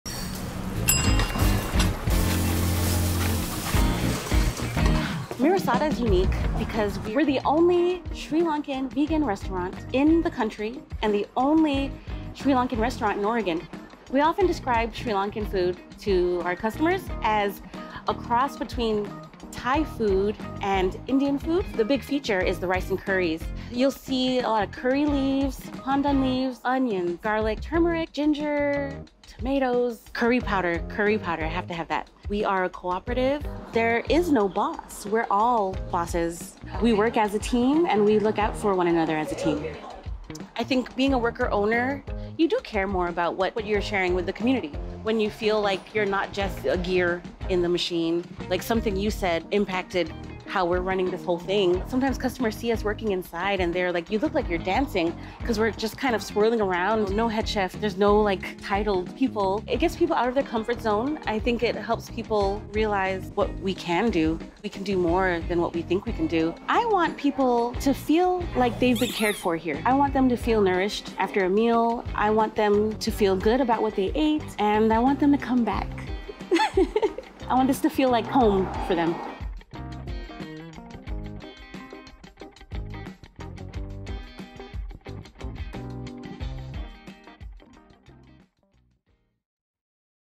Discover Mirisata: The only all-vegan Sri Lankan restaurant in the country! 😋🍛
Portland, Oregon is the home of Mirisata, the only all-vegan Sri Lankan restaurant in the country! Aside from their delicious food, Mirisata stands out by being a worker-owned cooperative that brings Sri Lankan family traditions and recipes to the kitchen.